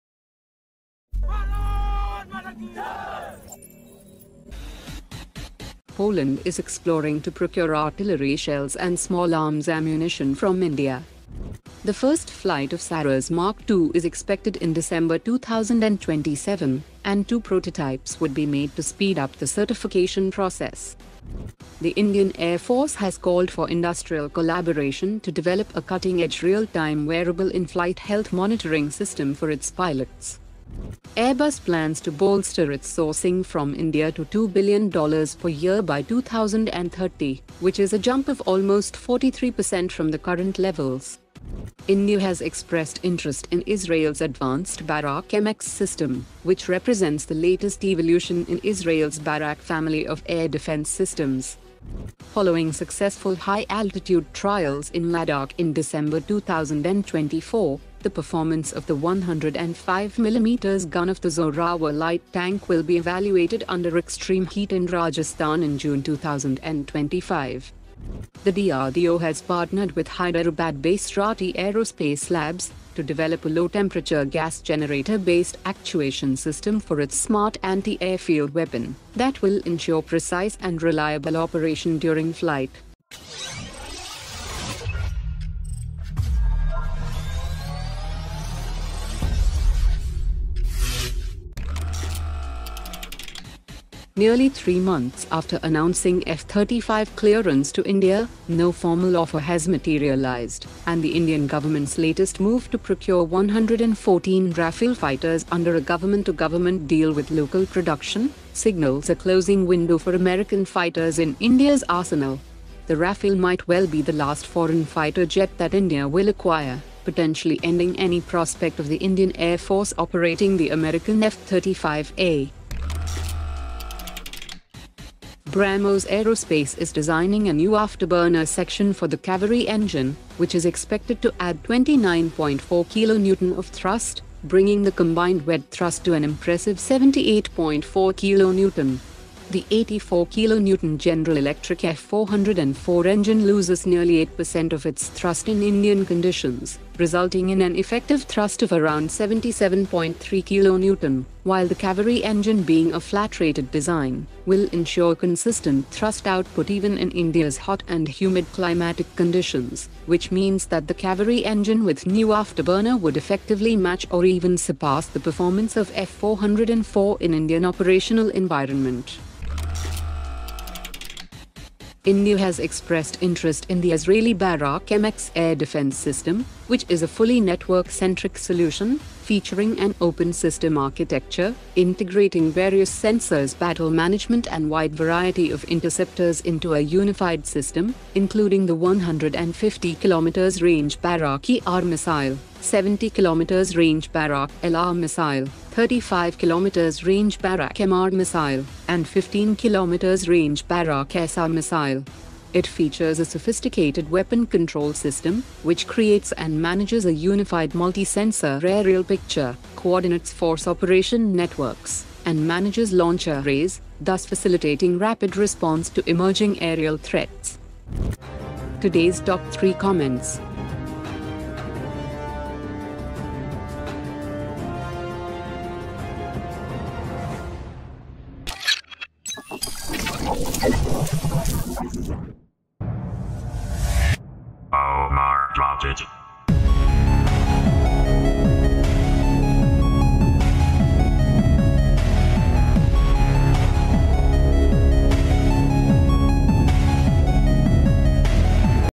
Indian Defence Updates : F35 Deal Closed,78.4 Kn Kaveri Engine,Barak MX Order,Poland Artillery Order
🔴 FAST HEADLINES :
1. Poland is exploring to procure artillery shells & ammunition from India
2. First flight of Saras Mark2 expected in December 2027
3. IAF seeks industrial collaboration for Remote Real-Time In-Flight Health Monitoring of Aircrew
4. Airbus plans to buy $2Bn of parts from India every year
5. India Interested in Barak-MX Missile Defence System
6. Zorawar Light Tank gears up for Rajasthan Desert Firing Trials
7. DRDO and Rathi Aerospace Labs Join Forces to develop Low-Temperature Gas Generator for SAAW

🔴 DETAILED HEADLINES :
8. Window for F-35A sale to India closed : Govt prioritizes 114 Rafale G2G deal
9. Brahmos Afterburner to bring KaveriEngine to 78.4 Kn Thrust
10. India Interested in Israel's BarakMX Missile Defence System